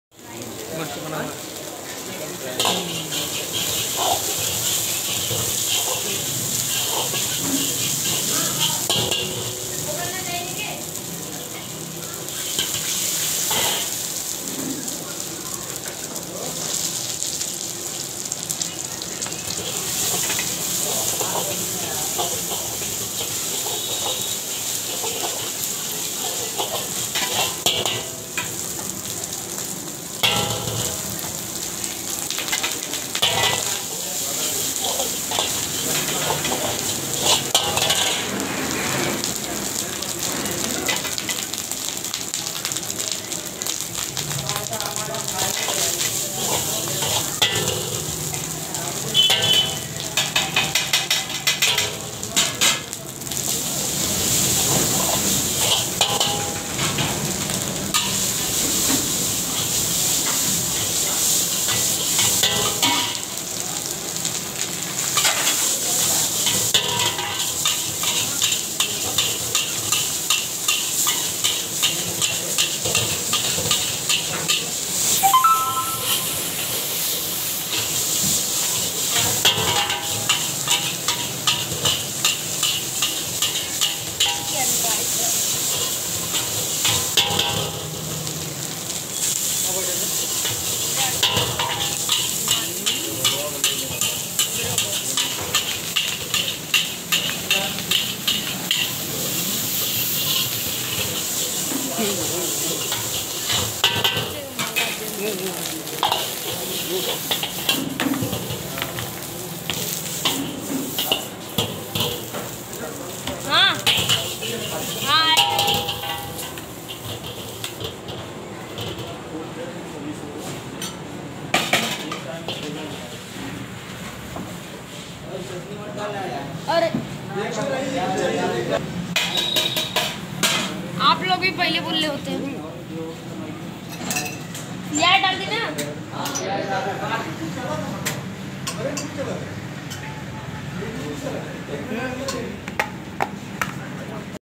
Chicken Fried Rice : Very Tasty Masala Rice For Rs 40 Chicken | Indian Street Food !
Restaurant style rice just in 2 Minutes Indian Street Food
Address - Jaunpur uttar Pradesh